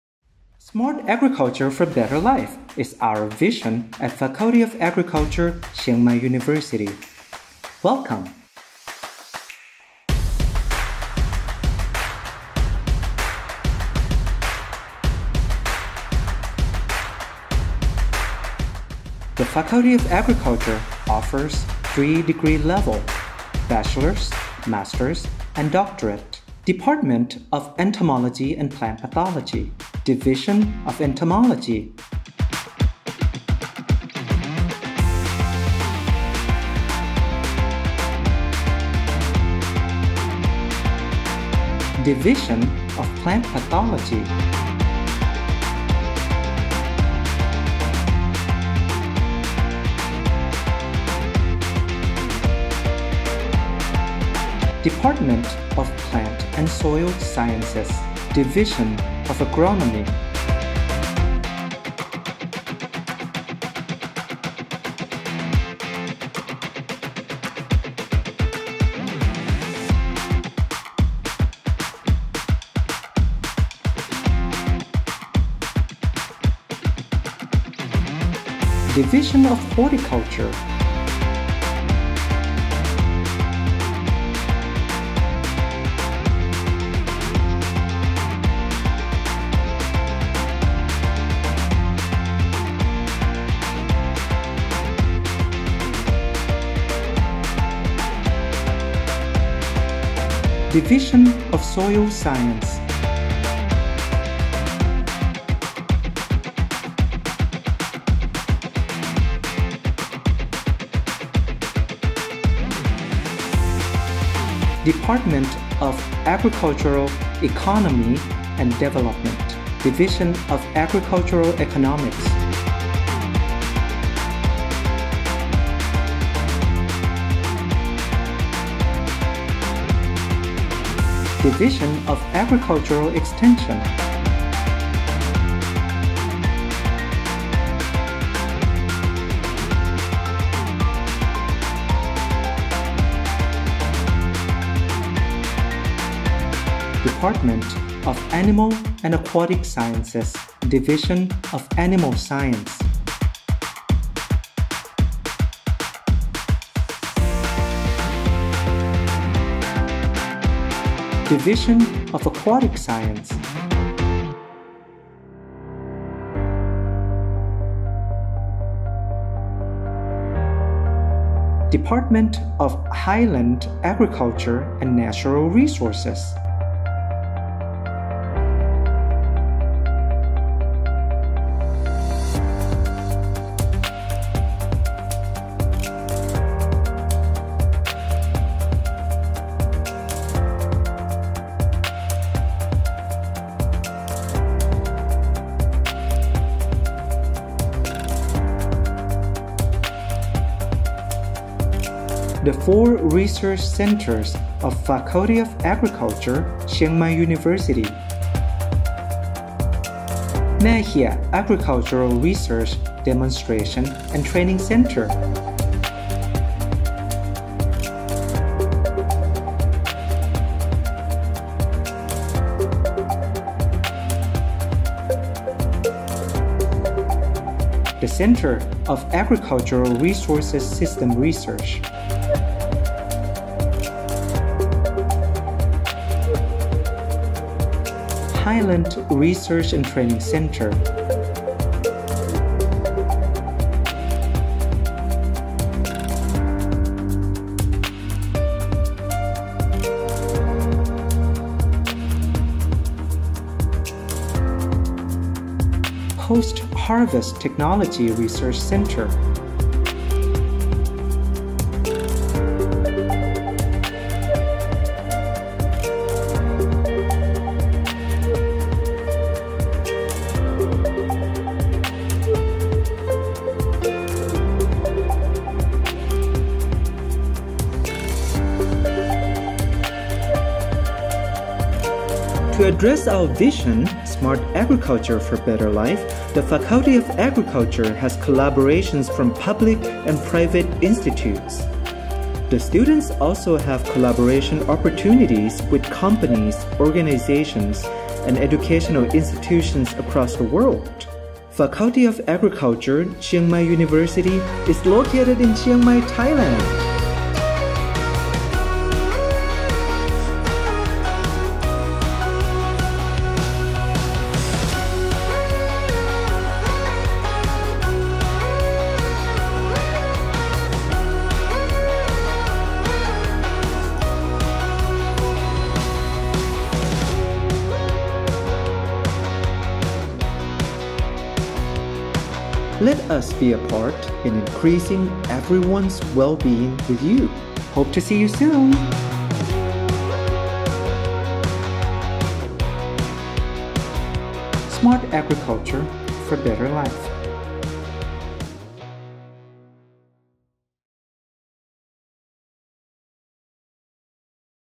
Pr Agri CMU international
Introduction for Faculty of Agriculture, Chiang Mai University and why we must study here !